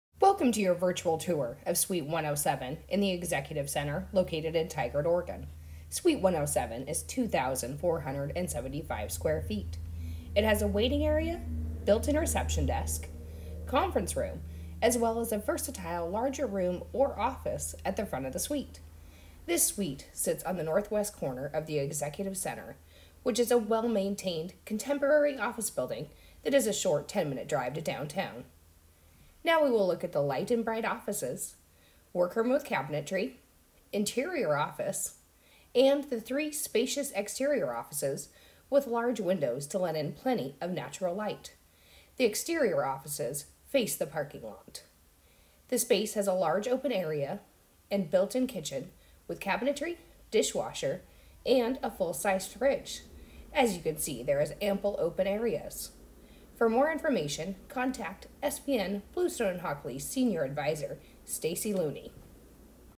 Executive Center: Suite 107 Virtual Tour
The Executive Center in Tigard, Oregon has available leases! The Executive Center is an attractive, well maintained contemporary office property. The well-appointed lobby features a sitting area, is elevator served and has an atrium stairway. The two-story skylit entry atrium welcomes you to the building. The skylights provide an abundance of natural sunlight. There are also restrooms on each corridor. Suite 107 is 2,475 square feet and includes many features such as conference room, built-in reception, window-lined offices and kitchen. Contact licensed Oregon Real Estate Broker, Stacy Looney with SVN Bluestone & Hockley, for more information.